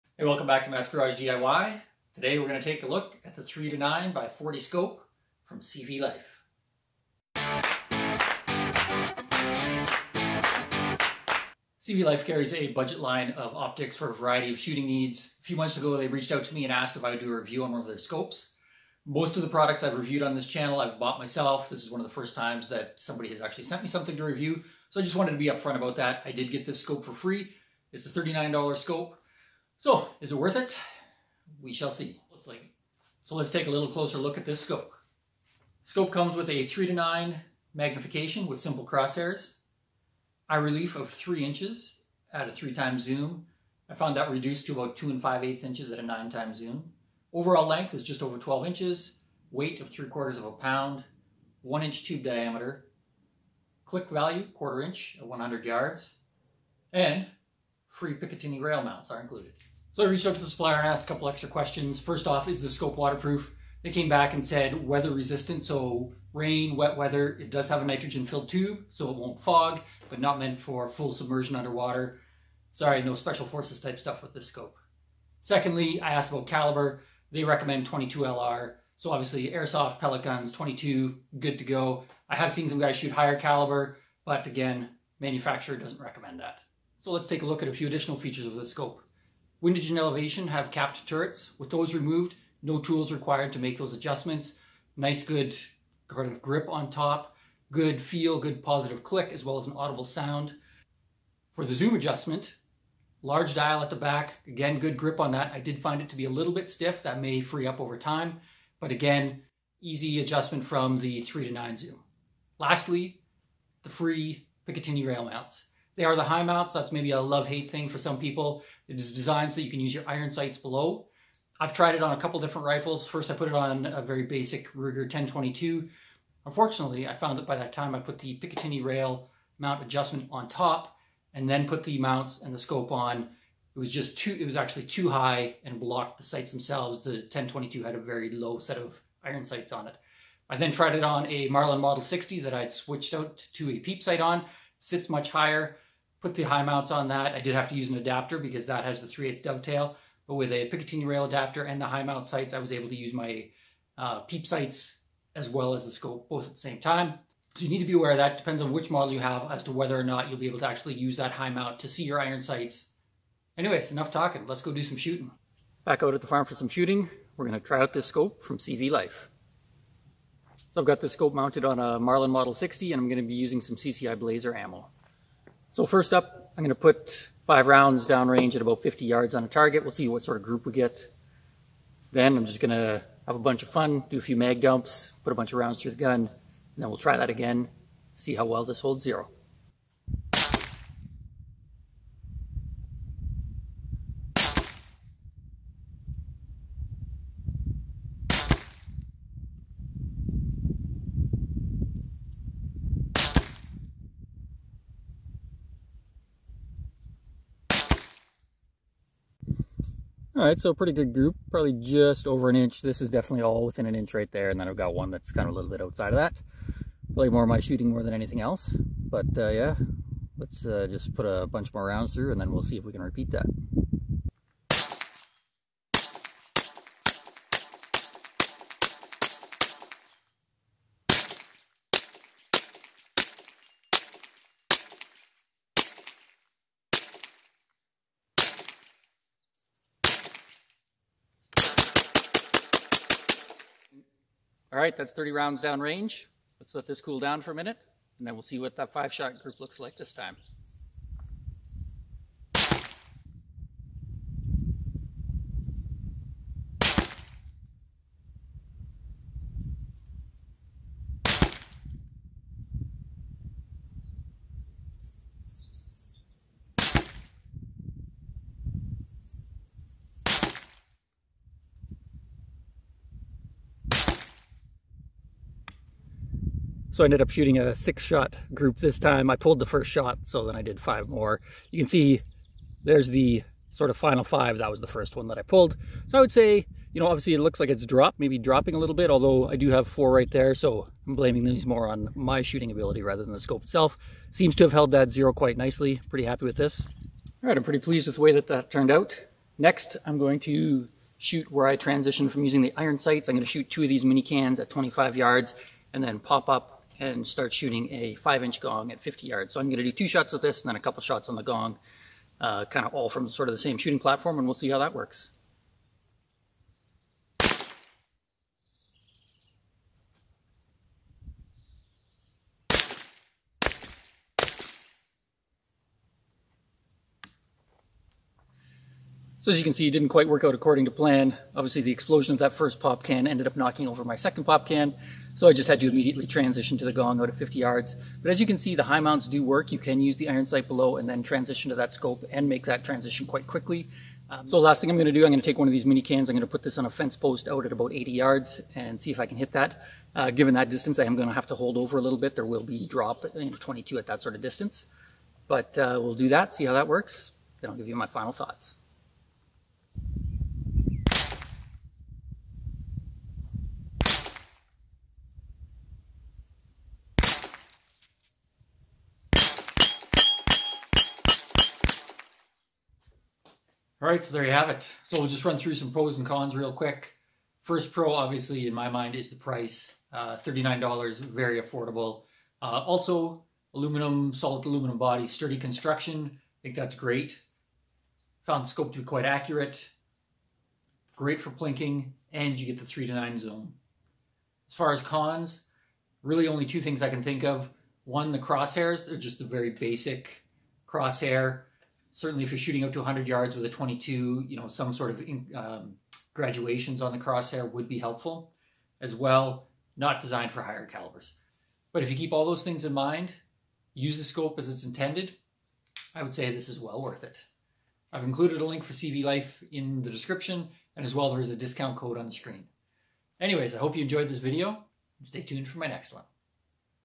CVLife - 3-9 x 40 Budget Scope review
In depth review of a budget scope from CVlife.  Is it worth it?  YOU be the judge.